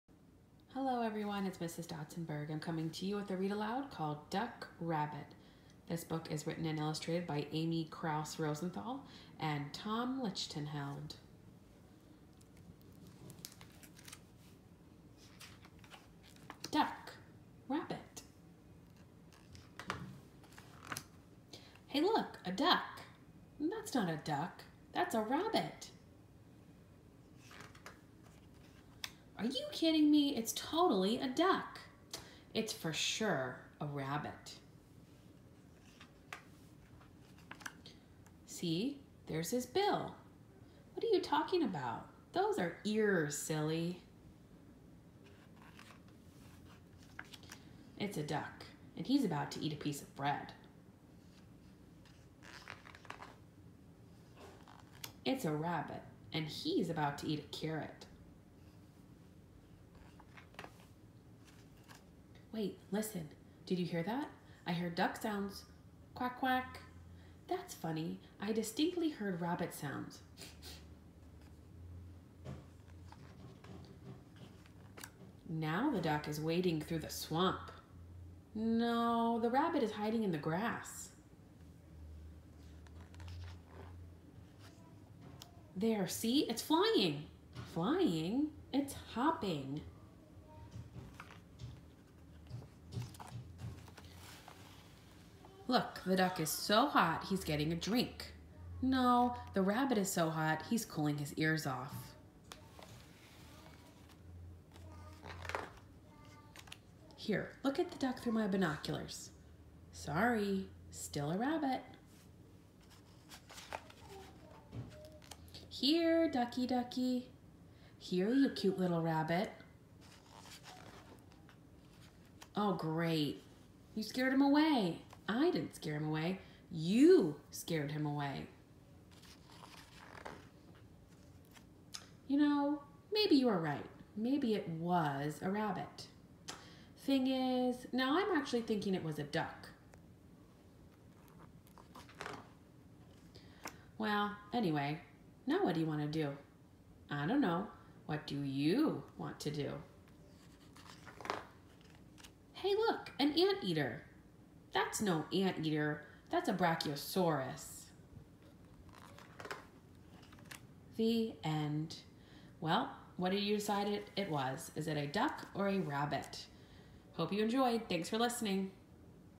Duck! Rabbit!--A Read Aloud By Mrs. Dautzenberg
Please enjoy this read aloud of the story Duck! Rabbit! by Amy Krouse Rosenthal and Tom Lichtenheld.